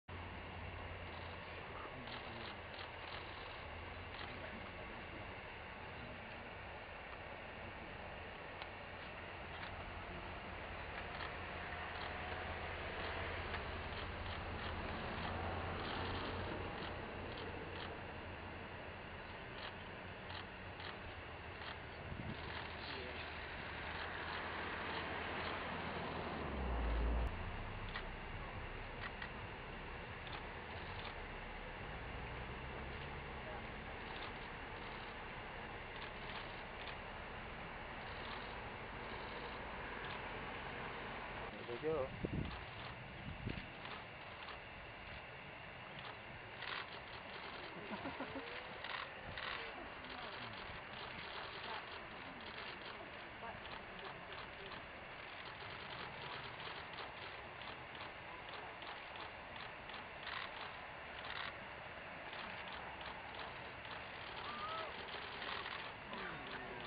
Bear Cubs playing in Lake Clark National Park, Alaska
Bear cubs doing some tree wrestling in Lake Clark National Park during a photography workshop with award-winning photographer Barrett Hedges. Video by Barrett.